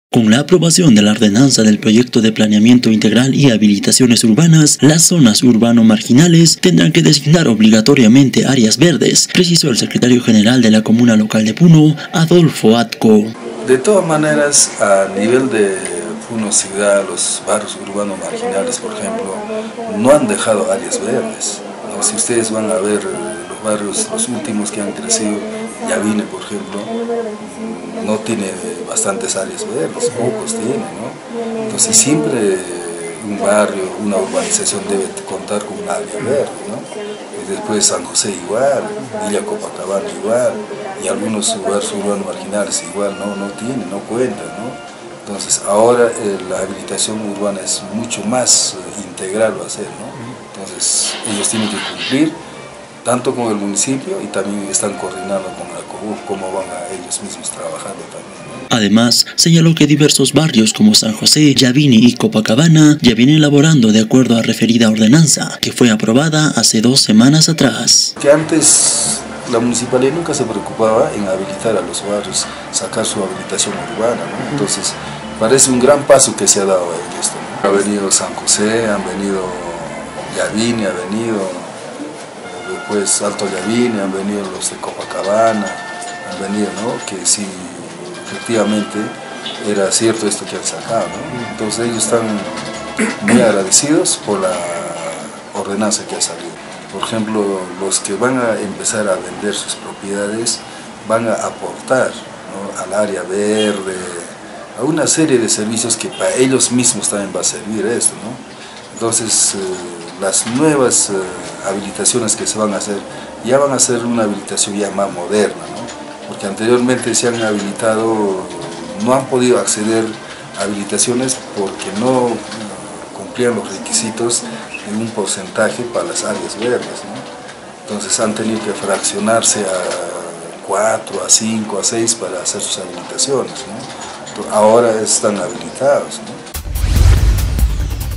ORDENANZA OBLIGA A QUE URBANIZACIONES ESTABLEZCAN ÁREAS VERDES
CON LA APROBACIÓN DE LA ORDENANZA DEL PROYECTO DE PLANEAMIENTO INTEGRAL Y HABILITACIONES URBANAS, LAS ZONAS URBANO MARGINALES TENDRÁN QUE ESTABLECER OBLIGATORIAMENTE ÁREAS VERDES, PRECISO EL SECRETARIO GENERAL DE LA COMUNA LOCAL DE PUNO, ADOLFO ADCO.